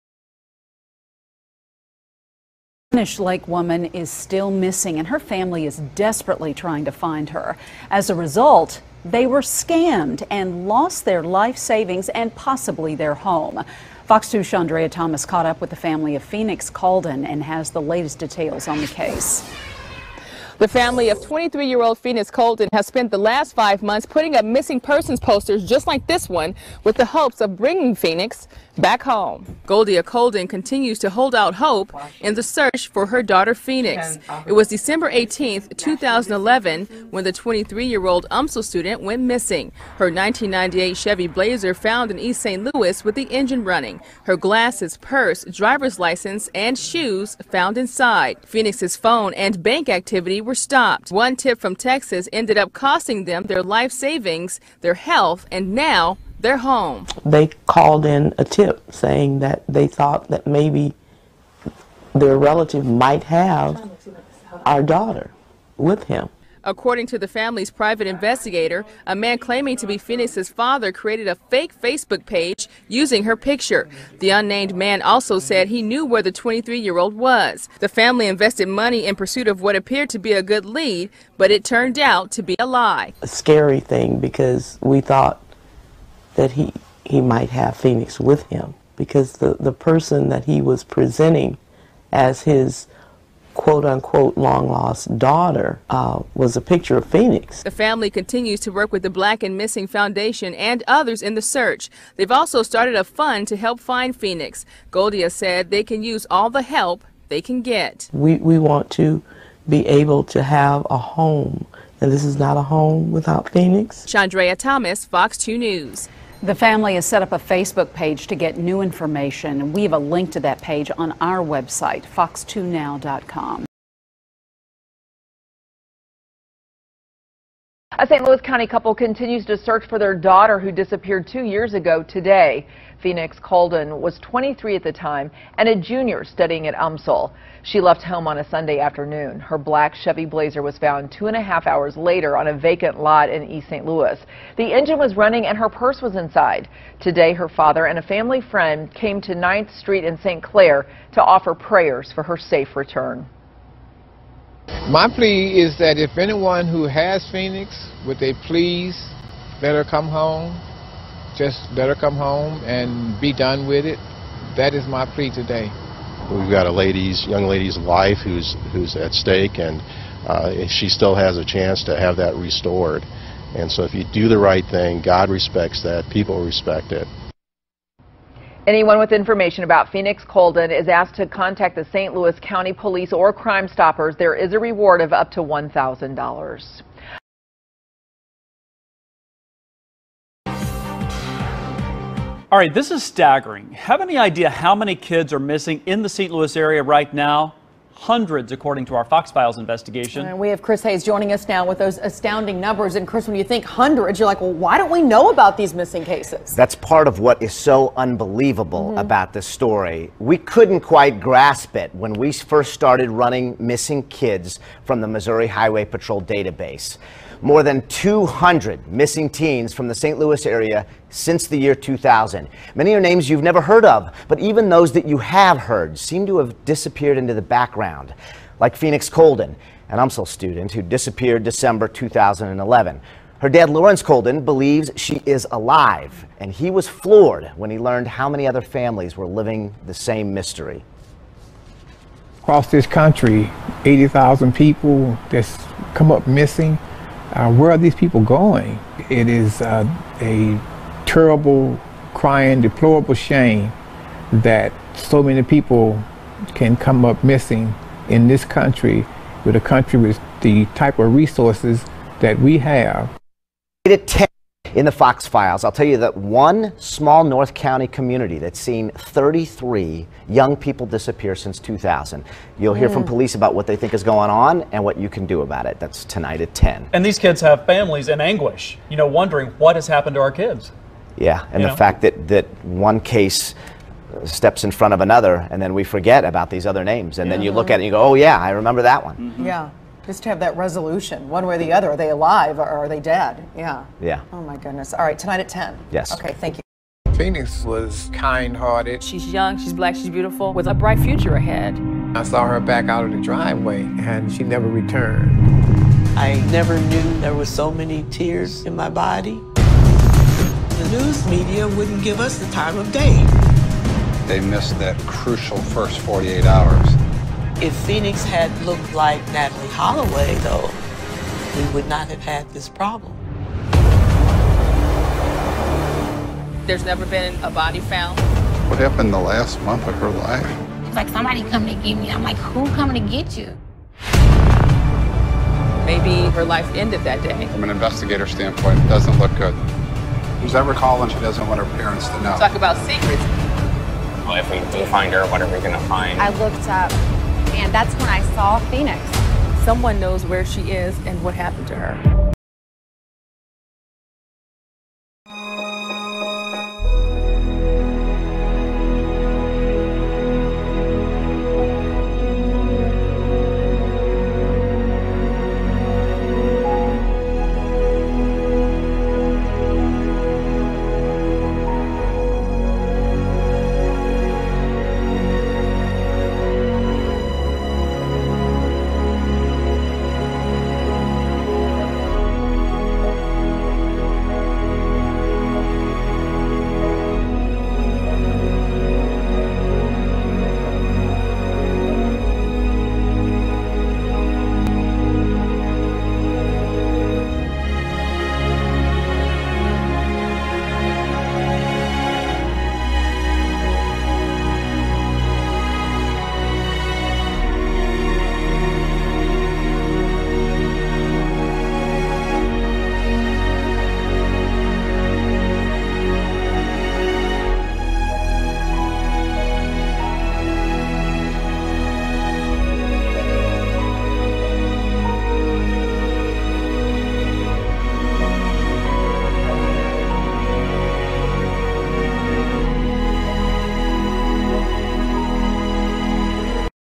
Phoenix Coldon attended Church on Sunday. . .went to her Spanish Lake Missouri home, and vanished.
Two and a half hours later Coldon's SUV was found, reportedly abandoned but running, in East St. Louis. Her glasses, purse, shoes, and ID were all inside the black 1998 Chevy Blazer, there was no sign of Coldon and the St. Louis Post-Dispatch added it wouldn’t be until Jan. 2 that a relative found the truck at an East St. Louis tow yard, the Blazer had been abandoned in the middle of the 900 block of St. Clair Avenue at 5:27 p.m. on Dec. 18. Phoenix's phone and bank activity stopped that same day in December. . .she has never been seen since.